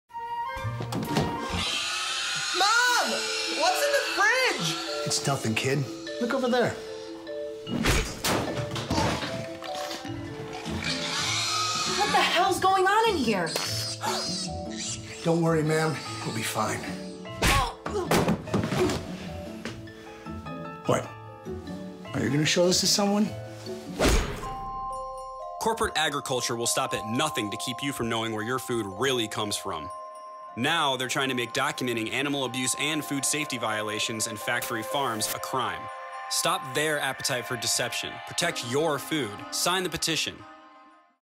What's in the Fridge?
Did you know food safety risks, animal abuse, and illegal working conditions are rampant on factory farms? And the corporate agriculture lobby is attempting to pass so-called "ag gag" laws -- laws which would make it illegal to photograph or videotape at agricultural facilities, or to possess or distribute such evidence. Their goal is insidious -- to quiet whistleblowers and eliminate access to critical evidence of farmed animal and other abuses. What's more, restrictions or prohibitions on these records would decrease the opportunities for food safety violations to be discovered, thereby increasing the risk that consumers could be exposed to and contract illnesses from the consumption of unsafe food products and produce from jurisdictions that pass ag gag legislation.

Learn why ag gag is of grave concern to everyone from journalists to whistleblowers to mothers concerned about the safety of their children's food, then see how the Animal Legal Defense Fund's proposed Protect Your Food Act can stop the threat of ag gag laws from reaching your state.